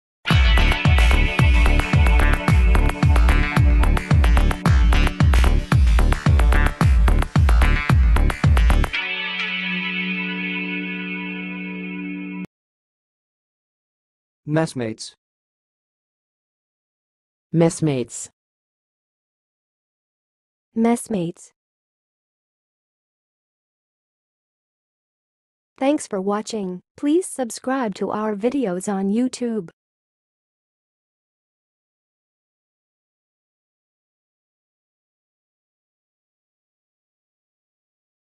MESSMATES - HOW TO PRONOUNCE IT?
MESSMATES - How do you really pronounce Messmates? Find out here in 2023

MESSMATES Meaning And Definition:
Messmates refers to a group of people who eat together, typically in a communal setting, such as a military mess or a ship's mess.
It can also be used more broadly to refer to close friends or companions who regularly eat together.
MESSMATES Pronunciation:
To pronounce messmates, say 'MES-mates'.
Other slight variations include 'MES-mates' and 'MES-mayts'.